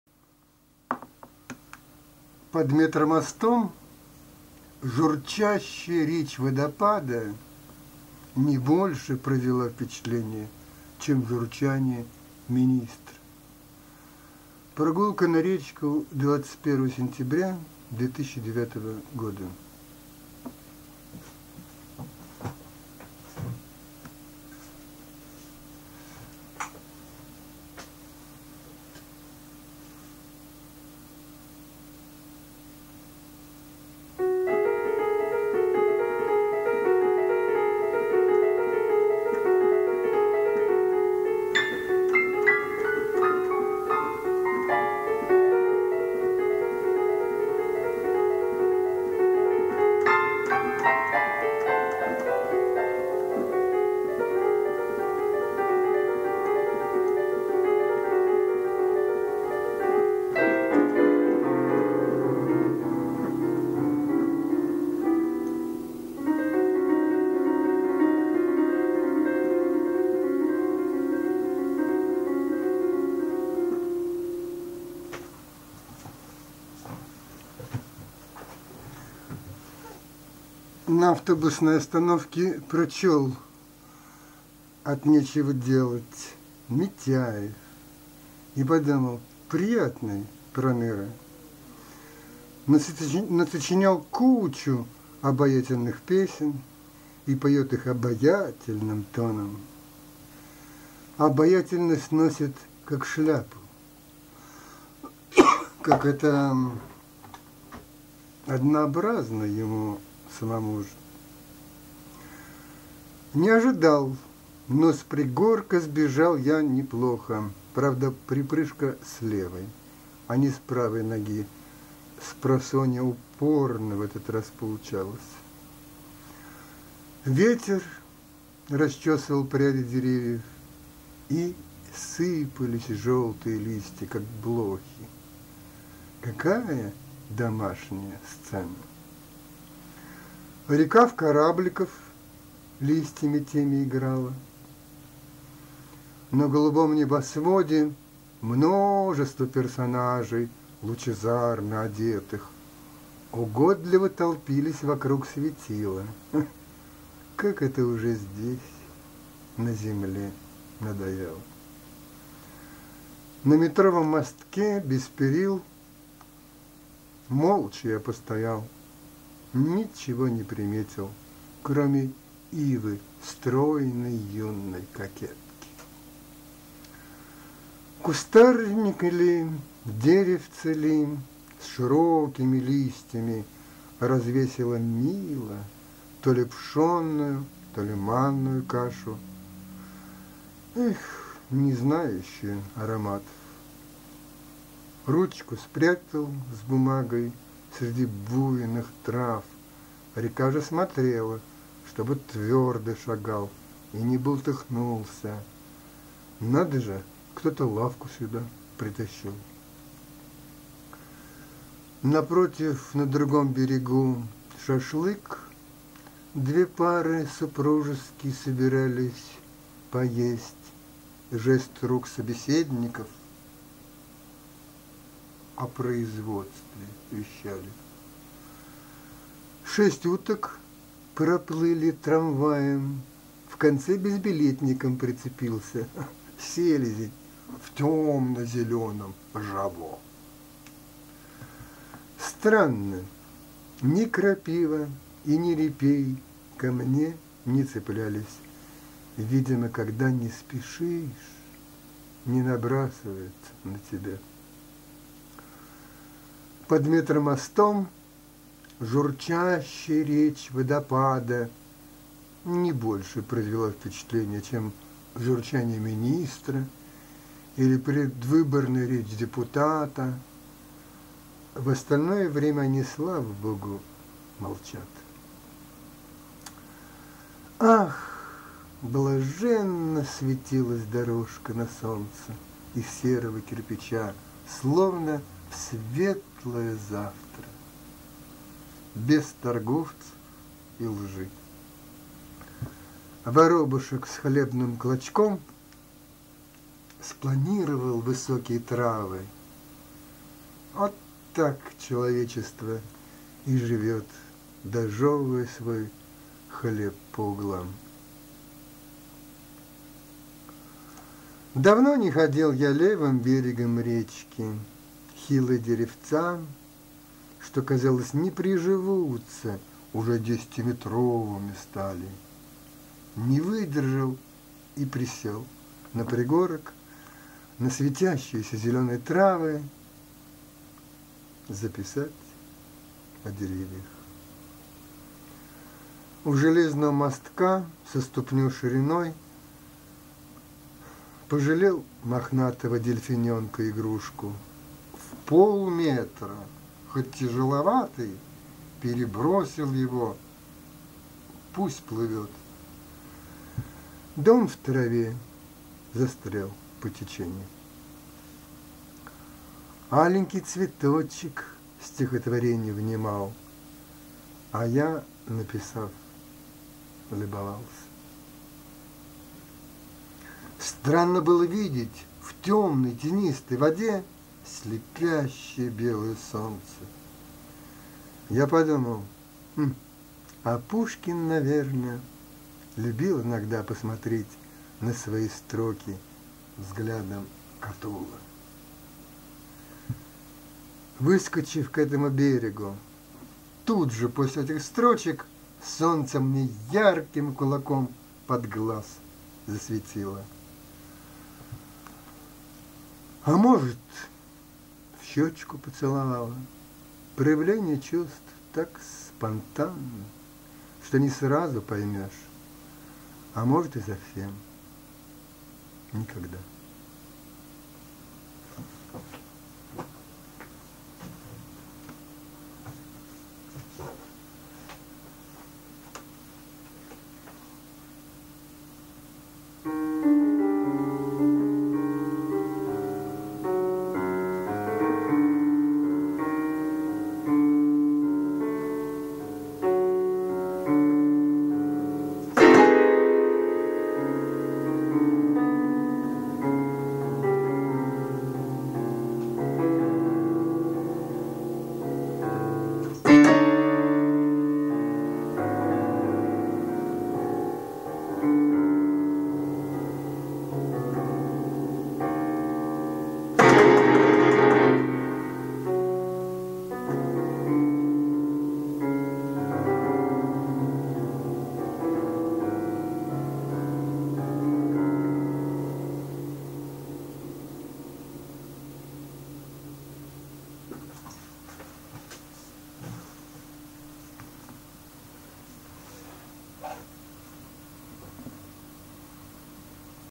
ЖУРЧАНИЕ водопада как речь МИНИСТРА
ТЕКСТ - стихи написанные во время прогулки пруду МУЗЫКА_чтение Юрий_Косаговский TV-Muzeum_Rondizm АРТ АВТОРСКОЕ_ЧТЕНИЕ видео-книга 2009